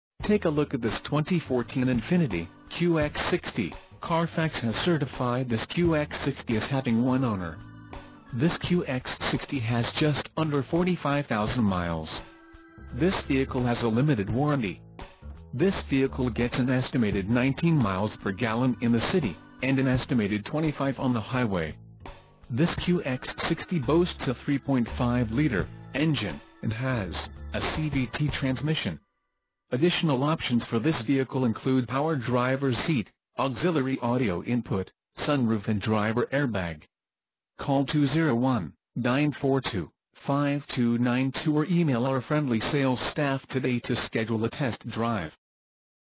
2014 INFINITI QX60 AWD 4dr SUV - Jersey City, NJ
New Jersey State Auto Auction, Jersey City, NJ - INFINITI QX60 AWD Leather Sunroof Reverse Camera 3rd Row Seating 1-Owner